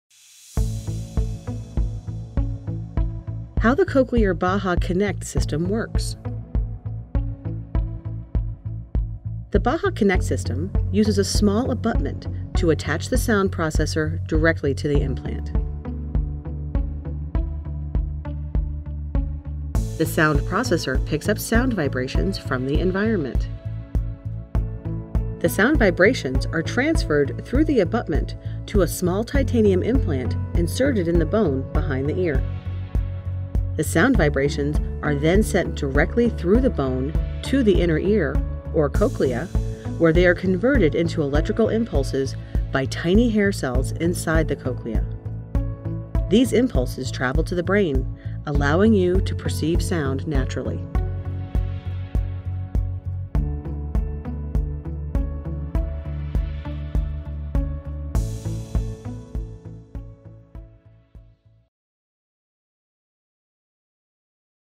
How the Cochlear™ Baha® Connect System works
Animated video of how bone conduction works.